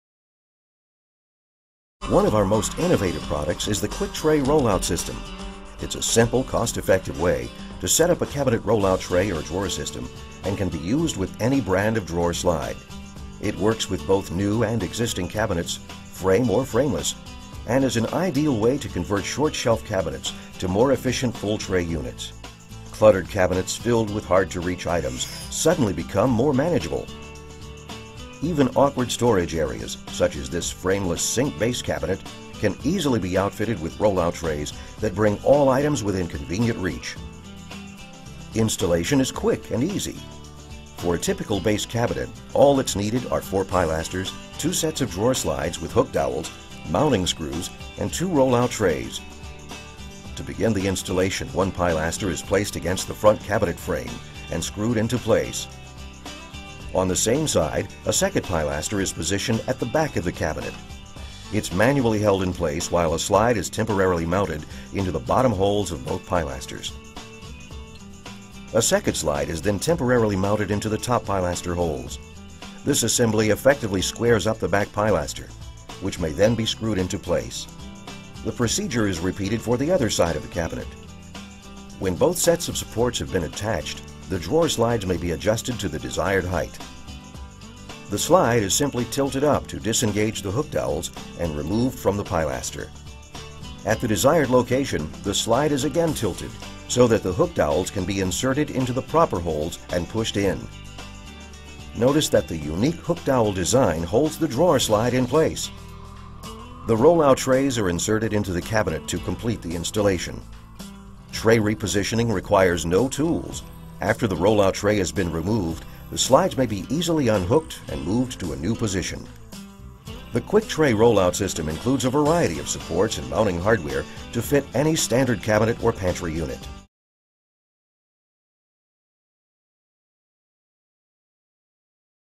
QuikTRAY™ Installation
Keystone Rollout Kits have everything you need including
• Dovetailed drawer box trays
• Drawer slides
• Tray bumpers
• and the QuikTRAY™ Rollout System.

The QuikTRAY™ Rollout System components are made in varied sizes to accommodate installation requirements.
• Cost-effective
• Easy to install
• Accommodates both undermount and side mount drawer slides
• Works with frame and frameless cabinets
• Install in both new and existing cabinets
• Adjustable without tools
• "Stackable" for pantry cabinets
• Available in White, Beige or Cream